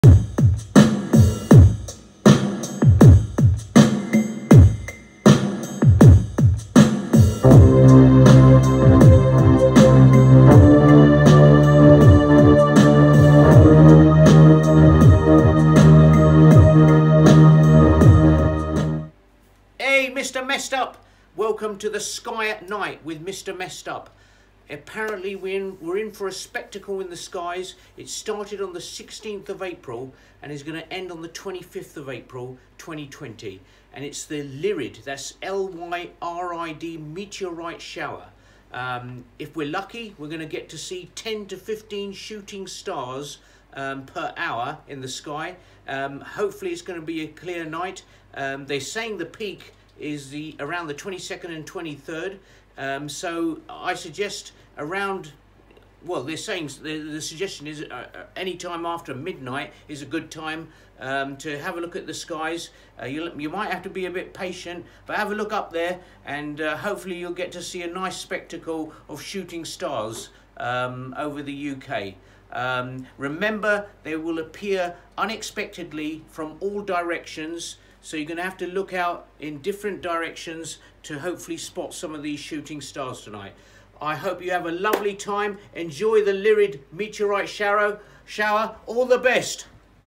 The sky at night with Mr Messed Up.The Lyrid meteor shower.Get star gazing.
Mr Messed Up discusses the Lyrid meteor shower visible from 16/4/20-25/4/20.We may see 10-15 shooting stars per hour.Peak days 22/4/20 and 23/4/20.Happy star gazing,cheers.

Credits-

Video of meteors from Pexel video.

Videography of mr messed up by A J F ilms London

Copyright of music in video remains with MESSED UP PROMOTIONS LTD U.K.(C)MESSED UP PROMOTIONS LTD U.K 22/4/2020